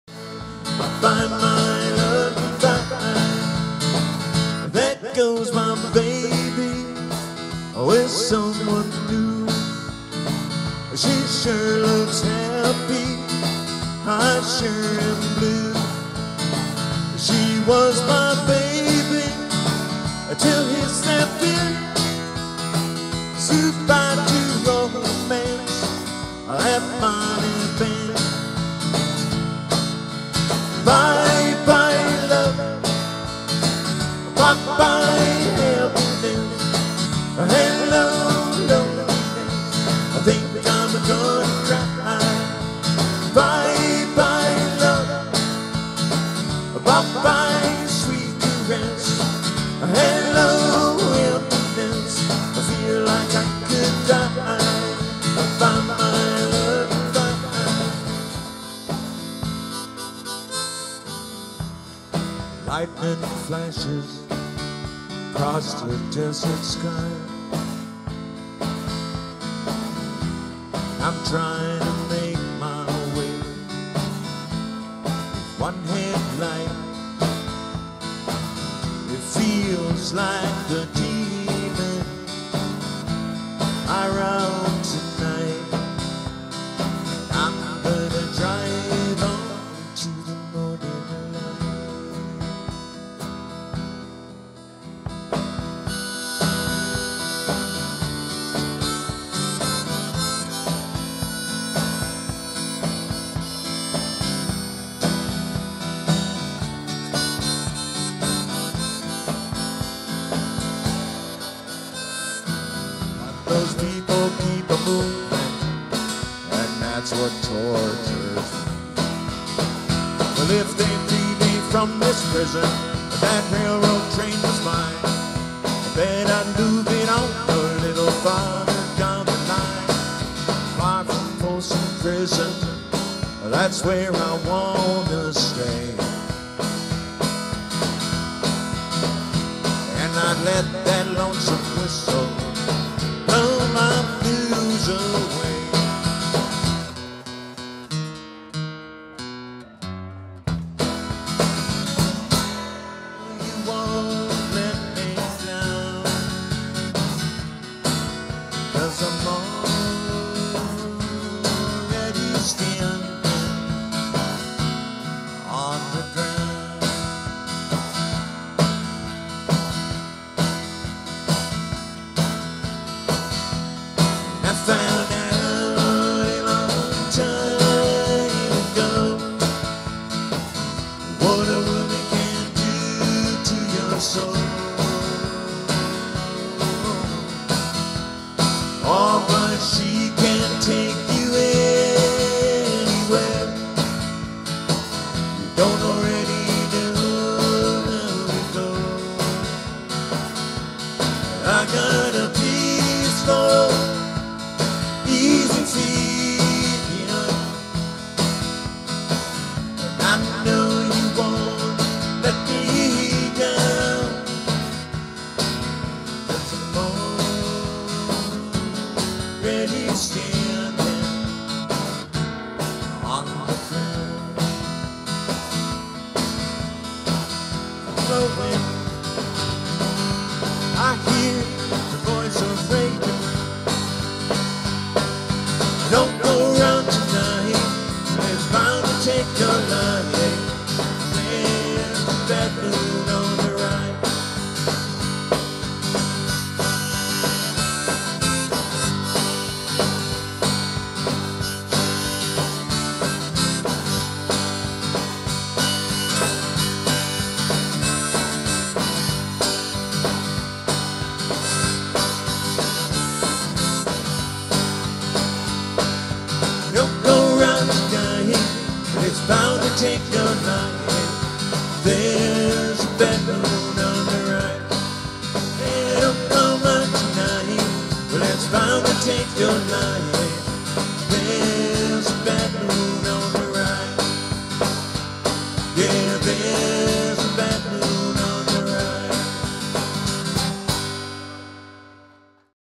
Grayling Pingel Music
A compilation of some of the songs Grayling Pingel performed at the Winter Whirl in Clintonville, Wisconsin on January 25, 2014.